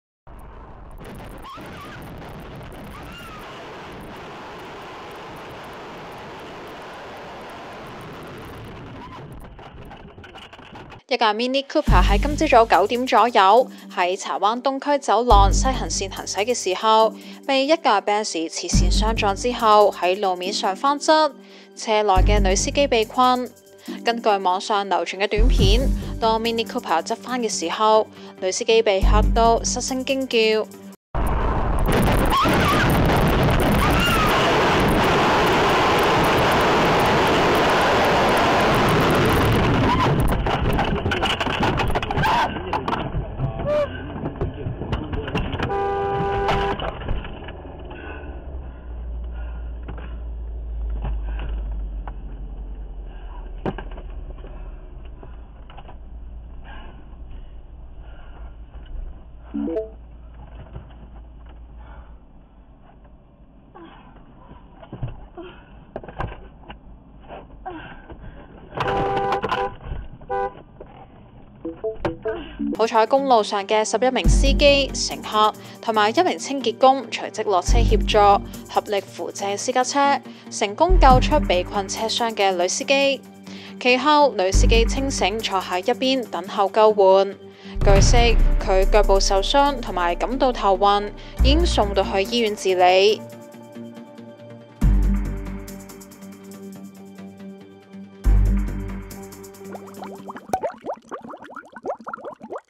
BENZ撞翻MINI Cooper 11熱心市民移車救被困女司機
睇片都感受到女司機的驚，好彩人冇事。